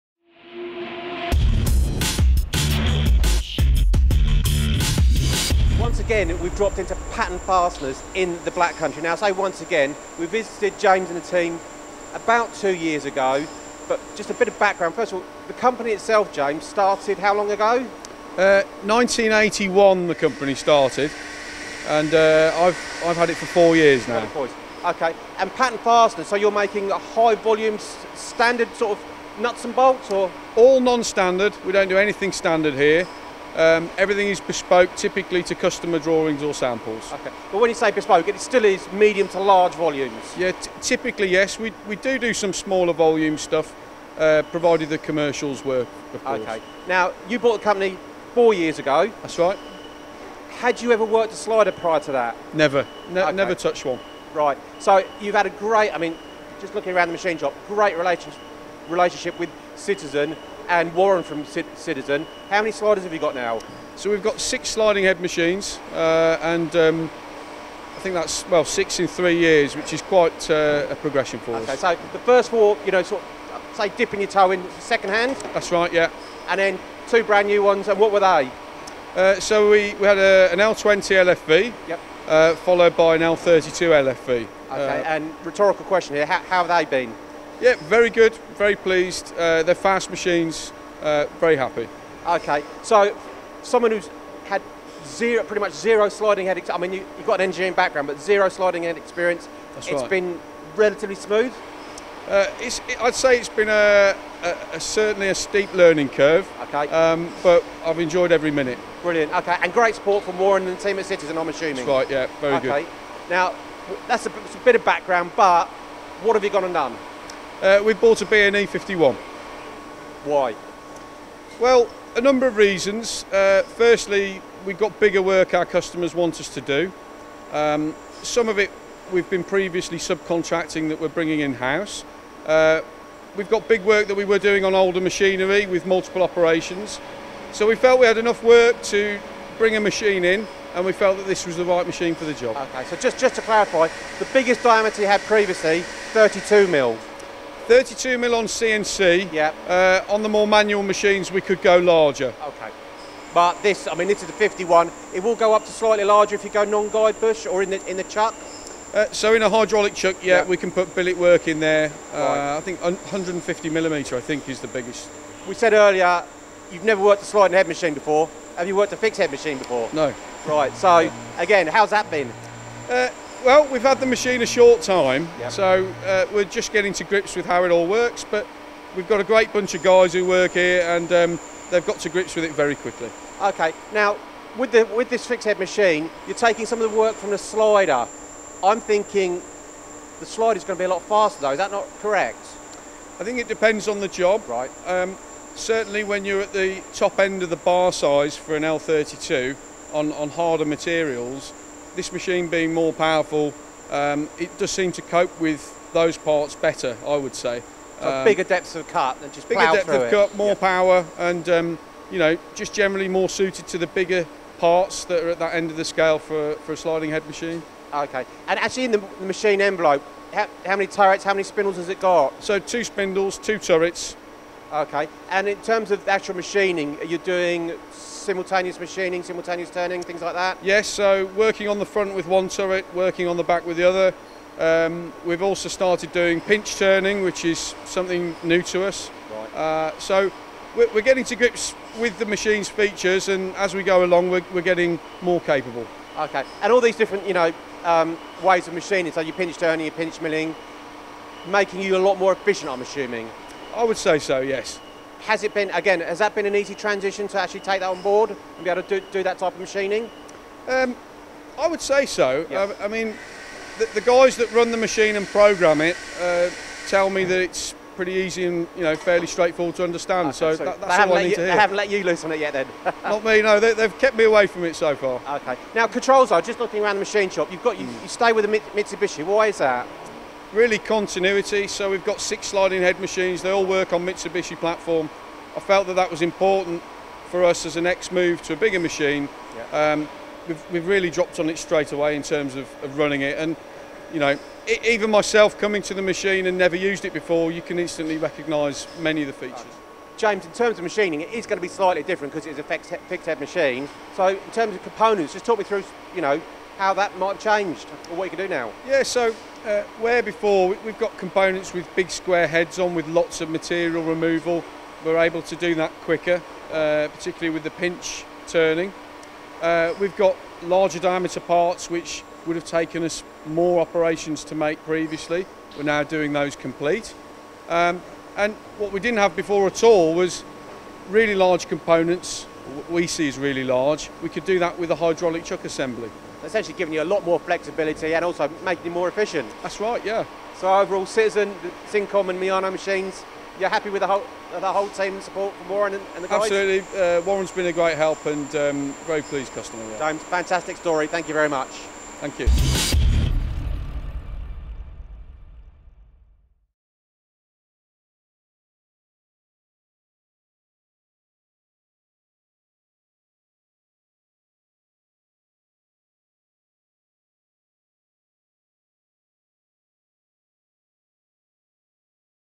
What Came First, The Slider or The Fixed Head
This is a great story, and here are a few numbers to share;
- 1981 - The Company Started
- 2016 - James Beddoes purchased the Company
- 2016 – They bought their first Slider
- 2020 – They bought their 6th  Slider
- 2020 – They Bought their first Miyano Fixed Head Lathe